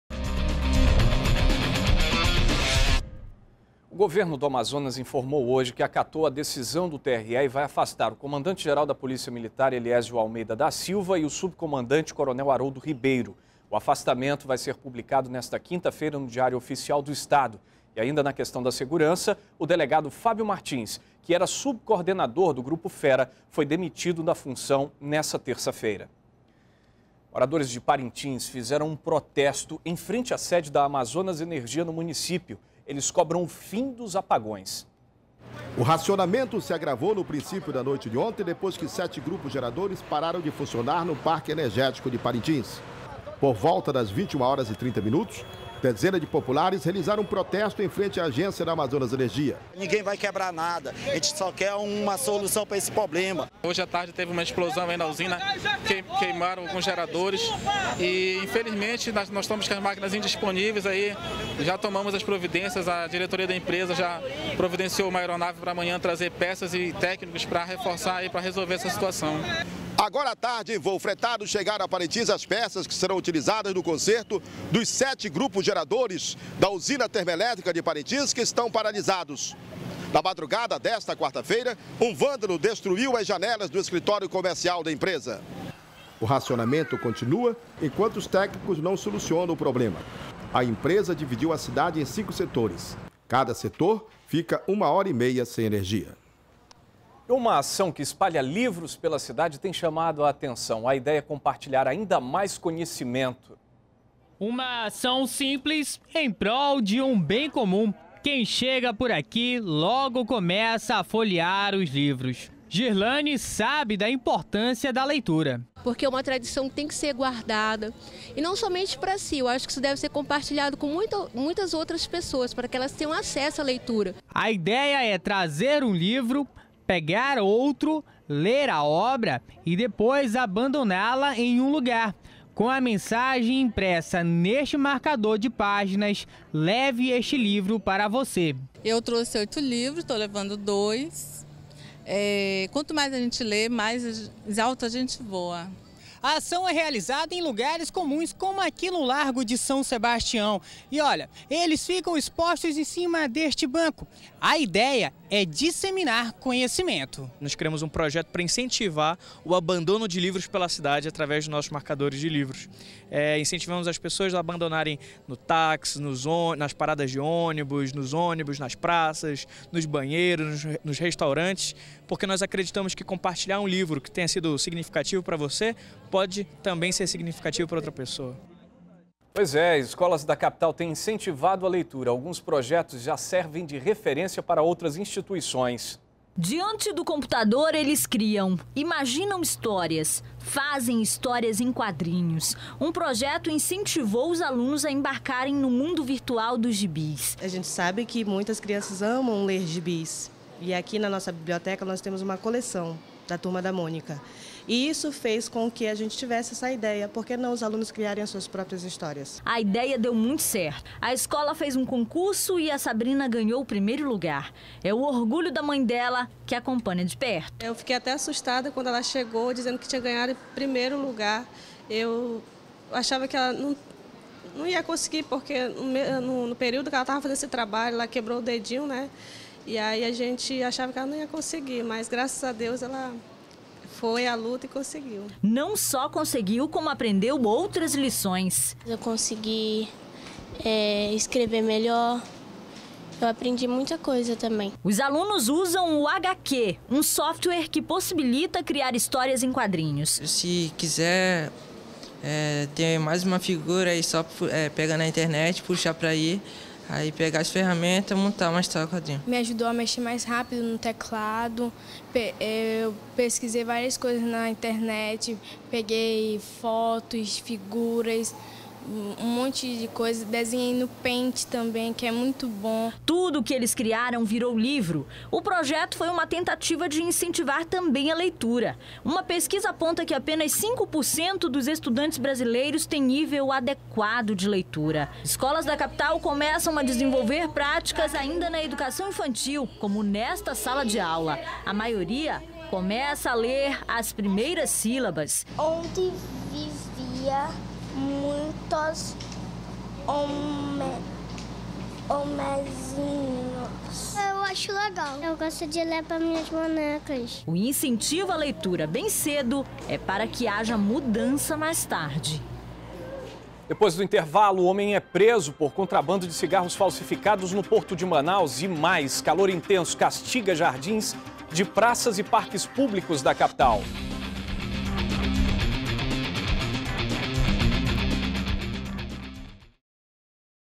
Moradores de Parintins/Am, fizeram um protesto em frente a sede da Amazonas Energia
02º BLOCO JORNAL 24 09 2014 - Moradores de Parintins/Am, fizeram um protesto em frente a sede da Amazonas Energia